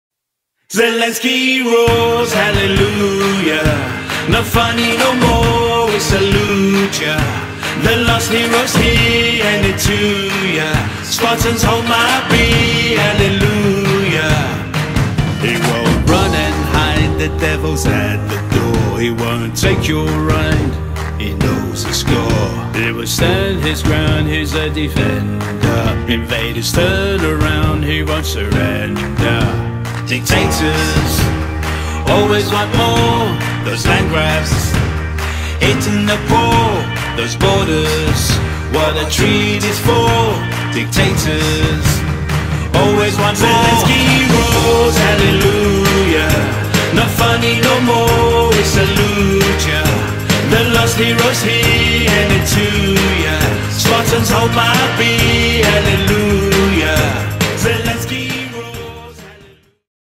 Zelensky Roars For Ukraine ( Slava Ukraini/ short version)
The tragedy of war committed by nasty dictators is clear.
Thousands of heroes emerge.
Not all are highlighted.
President Zelensky stands out as an extraordinary man.
Blessed with the gift of comedy, he has much more in his locker.